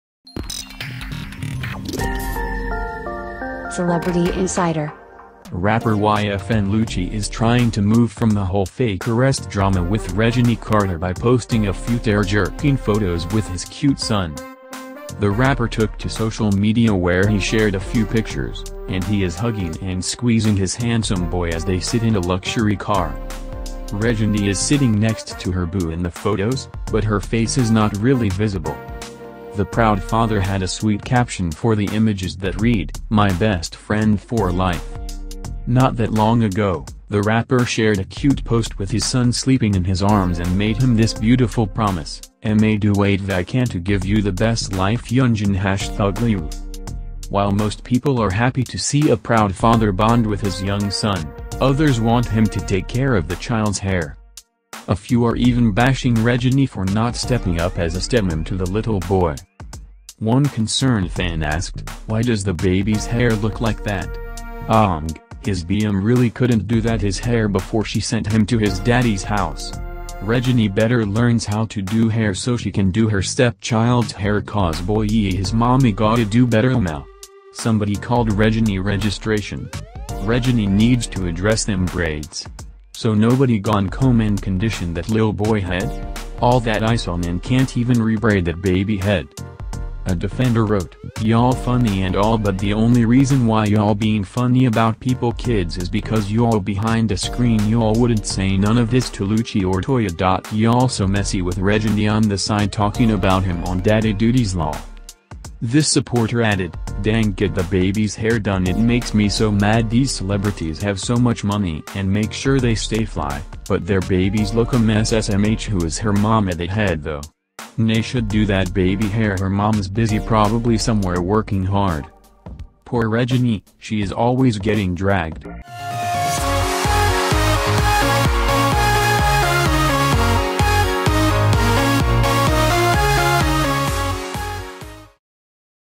Reginae Carter’s Boyfriend YFN Lucci Posts Cute Photos With His Son
Rapper YFN Lucci is trying to move from the whole fake arrest drama with Reginae Carter by posting a few tear-jerking photos with his cute son.